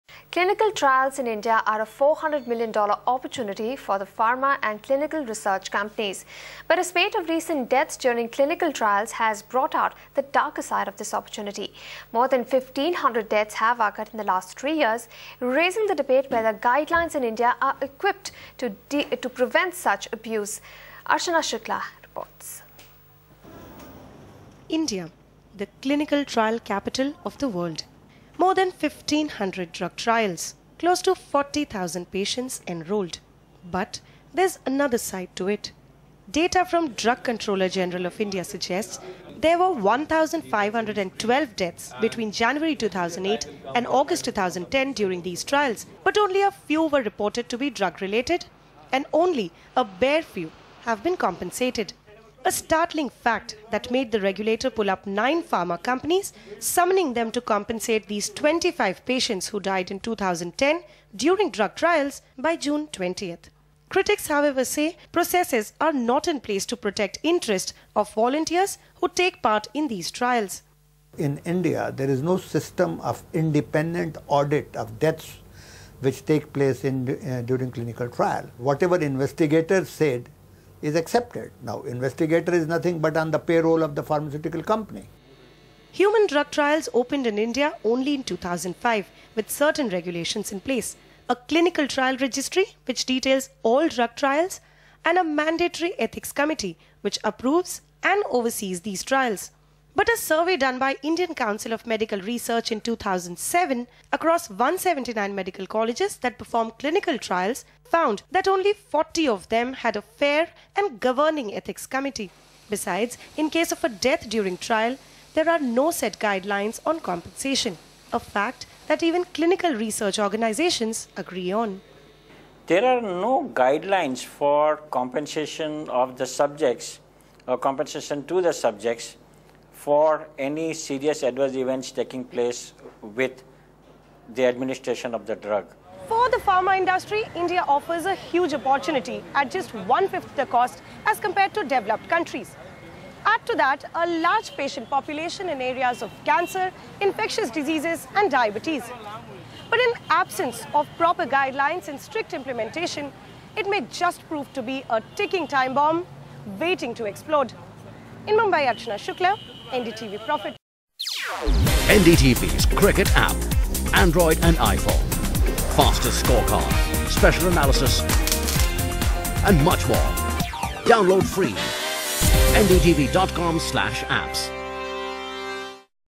Growing concerns over clinical trials
Clinical trials in India are a $400 million opportunity for pharma and clinical research companies. But a spate of recent deaths during clinical trials has led to regulator going tough on nine pharma companies by asking them to pay up 25 patients who died in 2010.